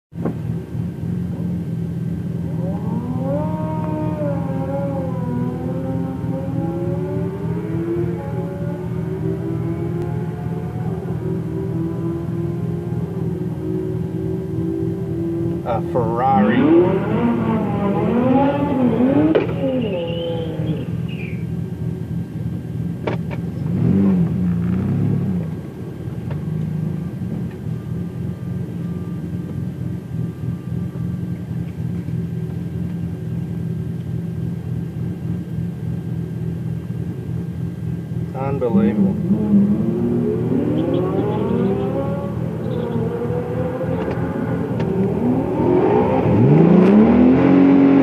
Ferrari 458 @ Willowbank Test N Tune Wednesday 9th August 2017
My veiw of the Ferrari from in the staging lane waiting for my run on the night.